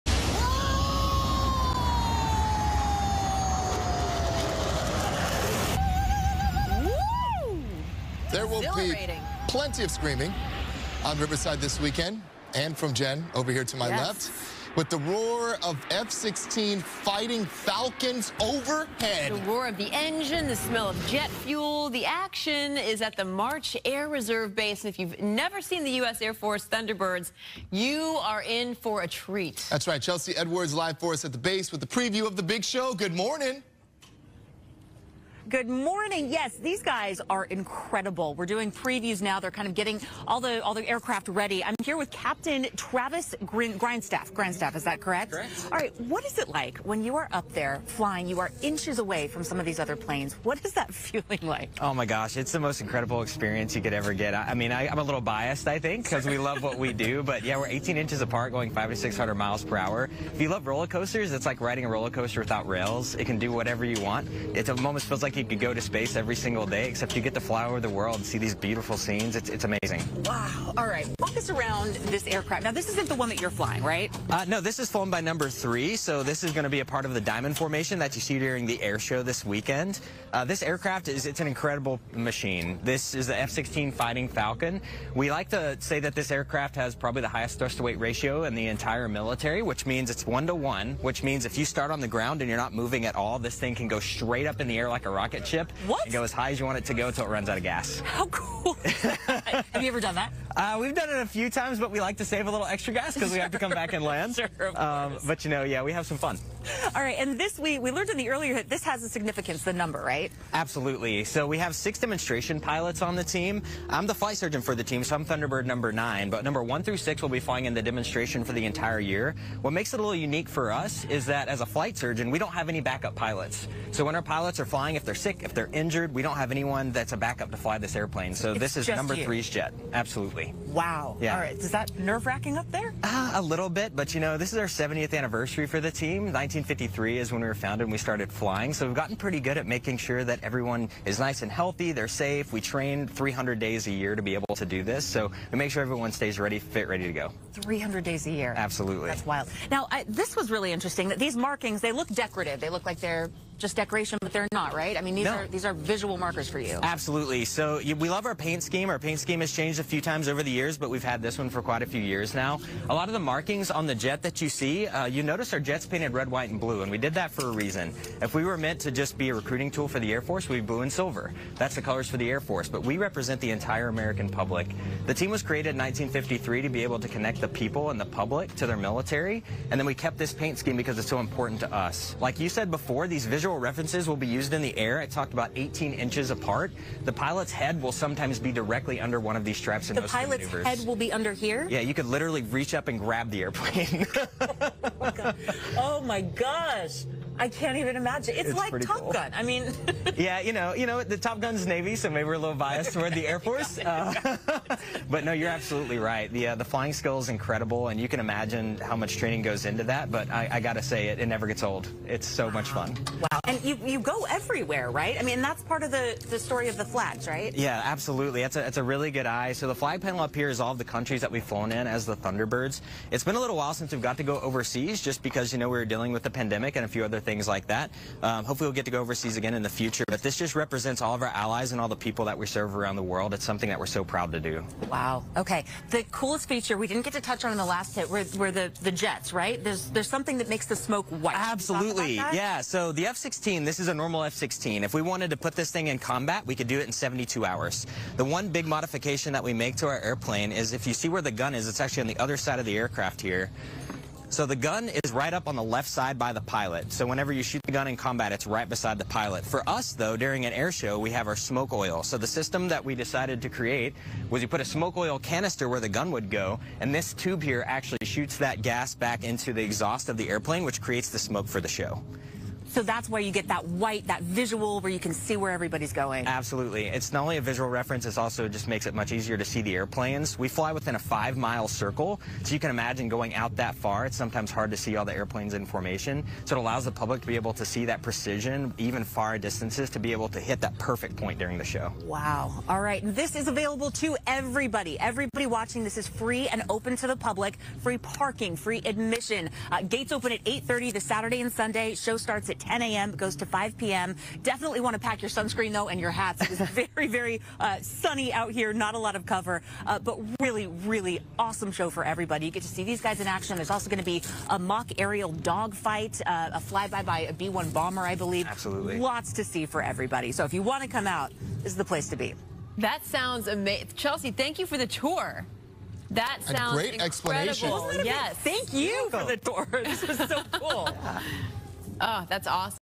SoCal Air Show returns to March AFB in Riverside
For the first time in five years, the Southern California Air Show is returning to the March Air Force Base in Riverside this weekend.